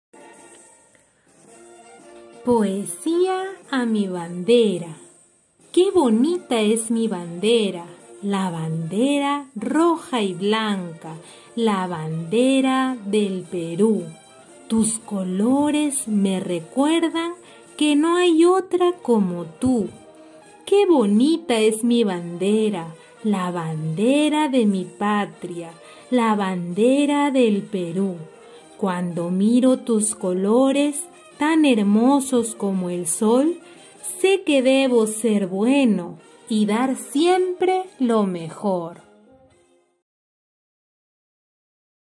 Día de la Bandera del Perú, 7 de Junio - Poesía corta para niños "A mi bandera" (Miss Tati)
A mi bandera

Qué bonita es mi bandera,
la bandera roja y blanca,
la bandera del Perú.
Tus colores me recuerdan que no hay otra como tú.
la bandera de mi patria,
Cuando miro tus colores tan hermosos como el sol,
sé que debo ser bueno,
y dar siempre lo mejor.

El 7 de Junio todos los peruanos celebramos el día de la Bandera del Perú. Conmemoramos así a nuestros valerosos hombres que, no rindiéndose ante el ejército chileno, se enfrentaron en Arica allá por el año 1880.